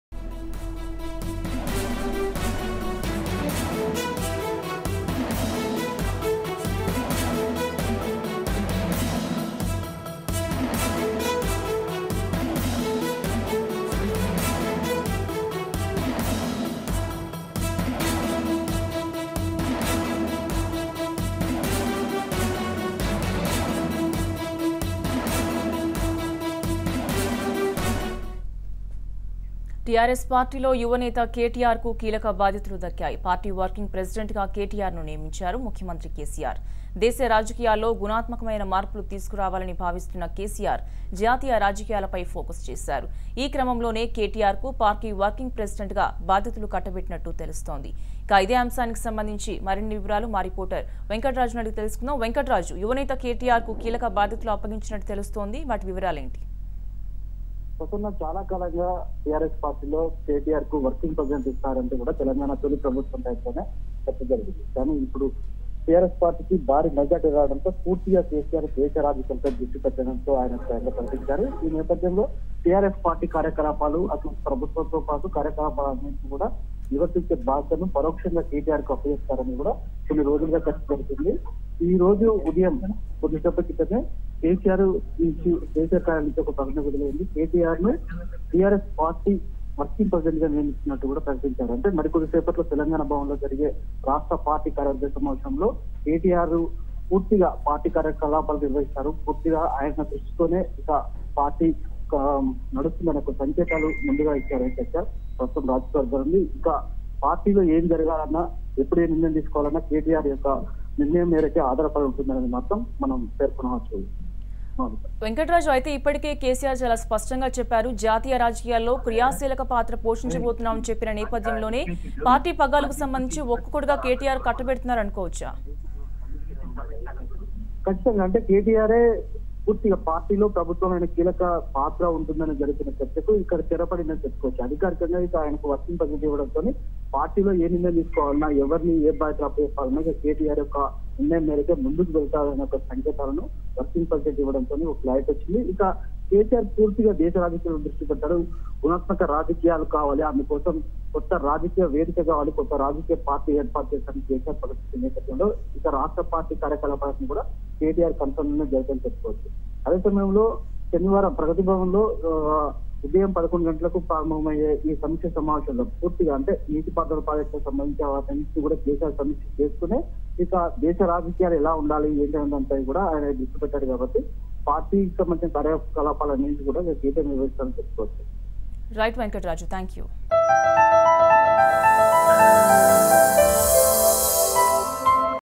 CM KCR Appointed KTR as TRS Party Working President | Raj News
CM KCR Appointed KTR as TRS Party Working President.